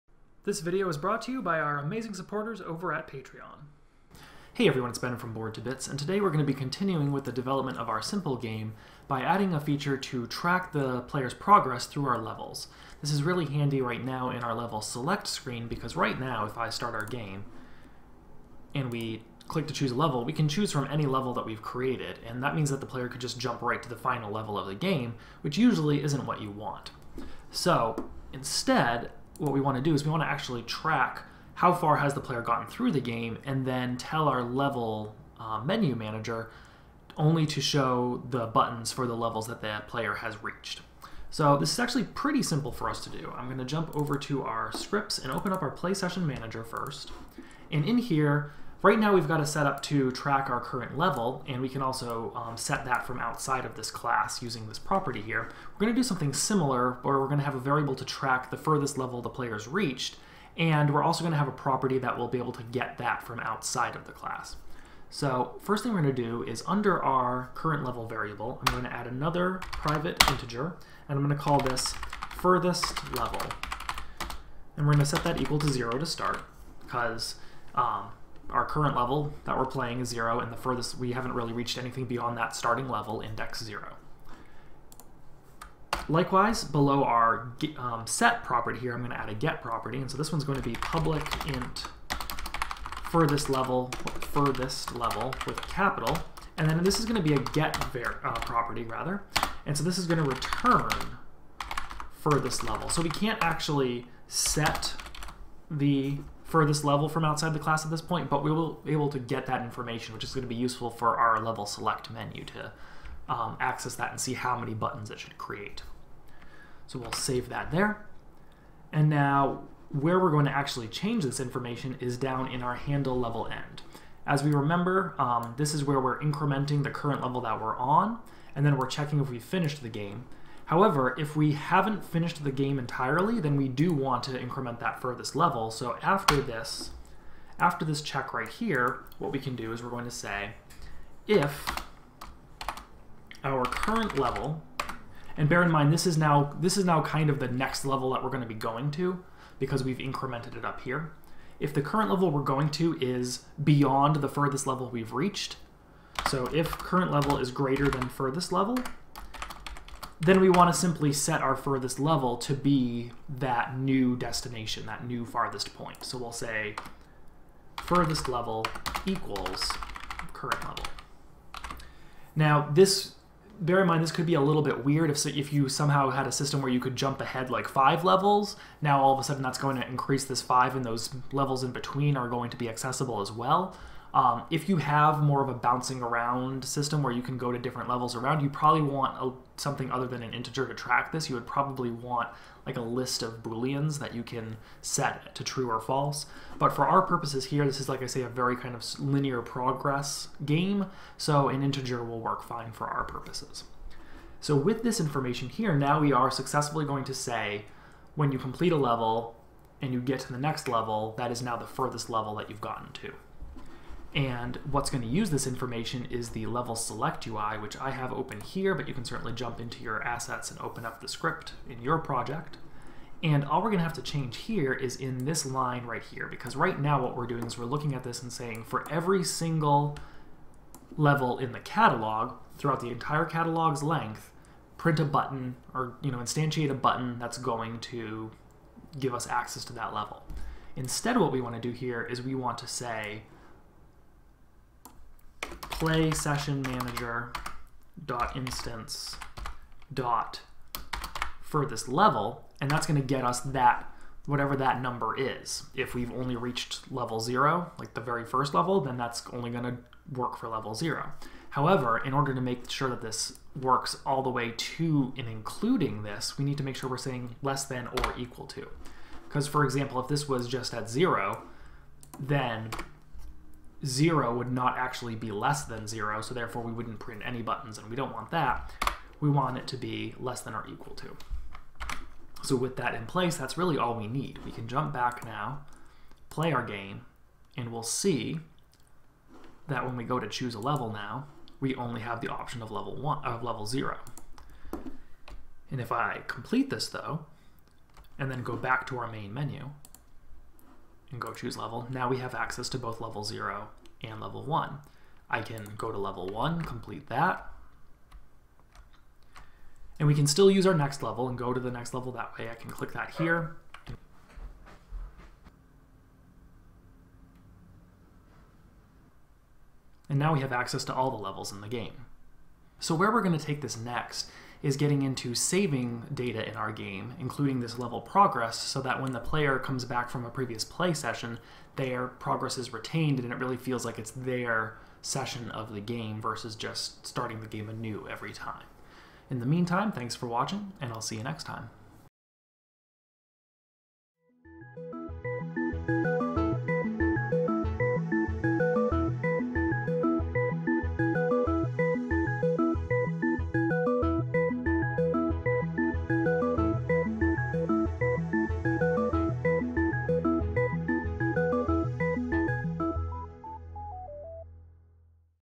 Non-Game Game Development: Tracking Progress
A simple setup to track player progress, and making the level select screen reflect that progress. Plus a tantalizing lead-up to saving game progress in future videos.